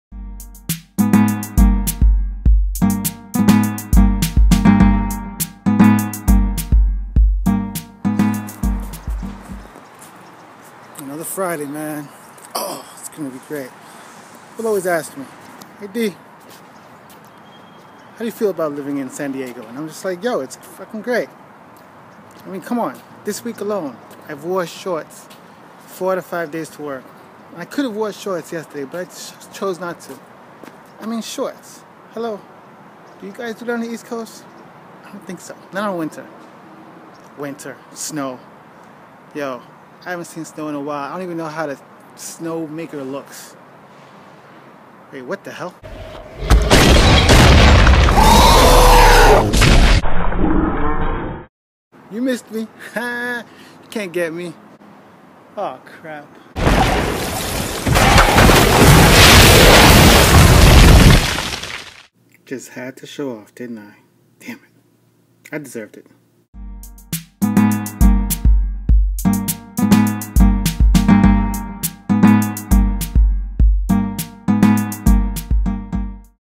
Snow Revenge Serenity
Episode 4 is here and all I can say is thanks to everybody that's watched, liked, commented and even shared these videos.

In this episode I'm answering the question that I get asked a lot which is "How do you like living in San Diego?" and well, I guess I got to cocky in my answer and I'm guessing my friends from the East Coast sent the Winter to get me.  Check it out.  lol

Music made by me in Garage Band.

All made and edited on my iPad.